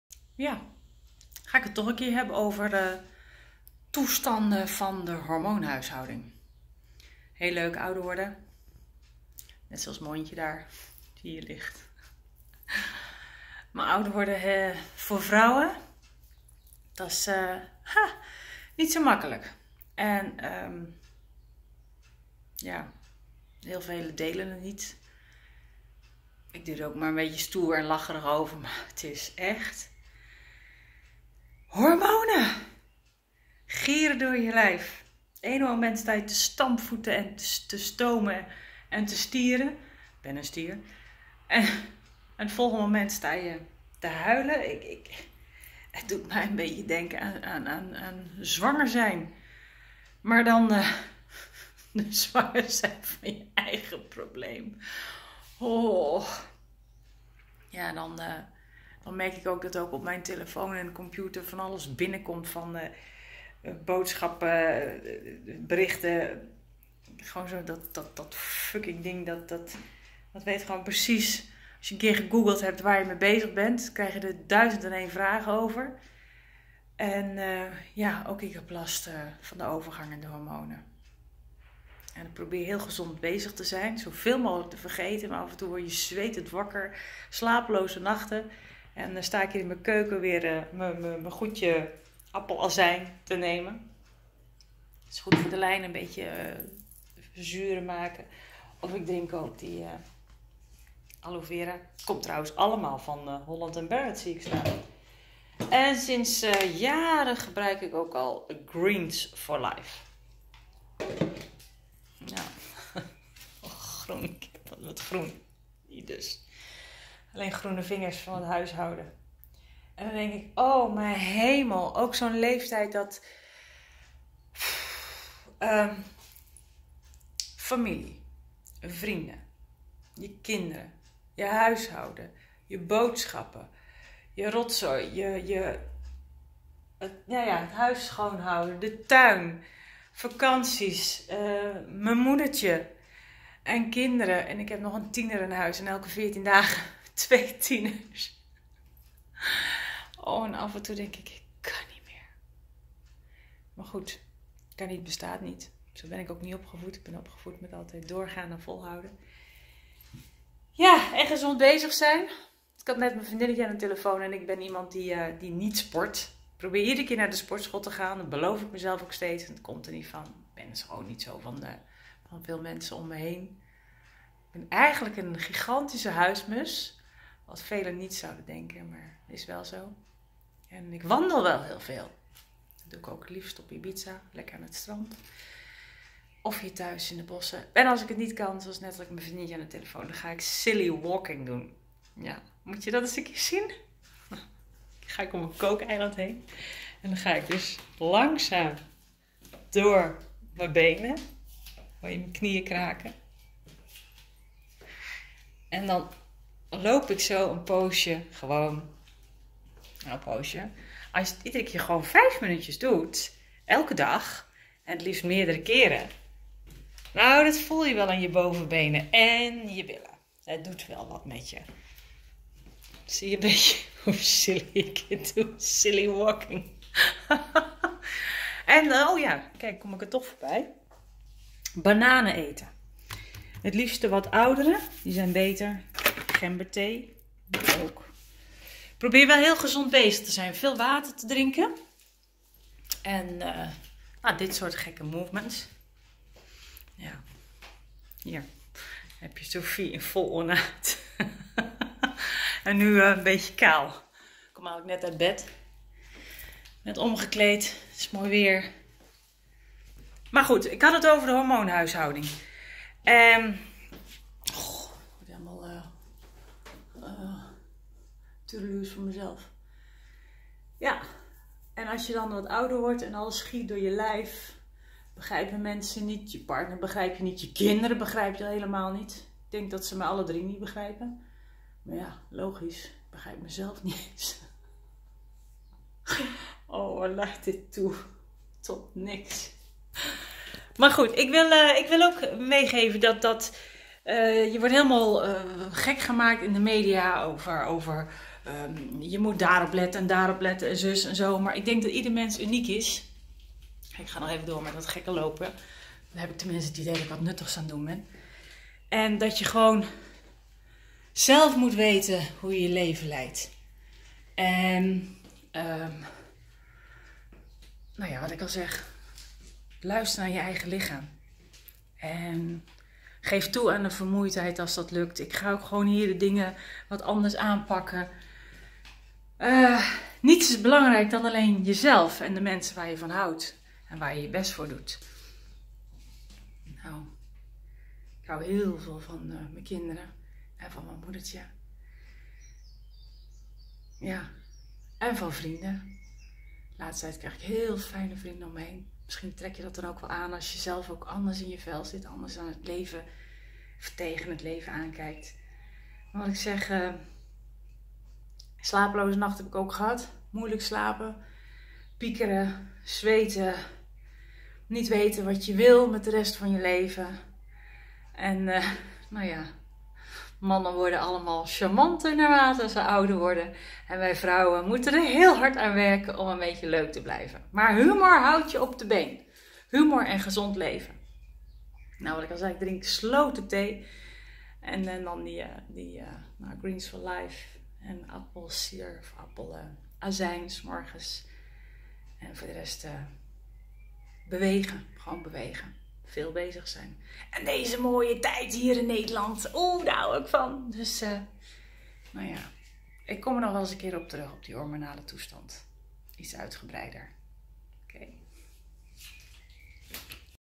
De HORMONEN-dans en SILLY walking? | als VROUW ouder worden | Sophia de Boer
Toch heeft (bijna) iedere vrouw het verdorie lastig met ouder worden... vreselijke hormonen en het dagelijkse leven wat ook doorgaat. Je werk en privé, gezin en pubers en je lijf die je in de steek laat! Alles veranderd zonder dat je daar controle over hebt. Pfffff wie oh wie zitten hier nog meer mee?? Ervaringen uitdelen real life is wel zo fijn... hellup de hormonen-dans is begonnen ook voor mij... 😣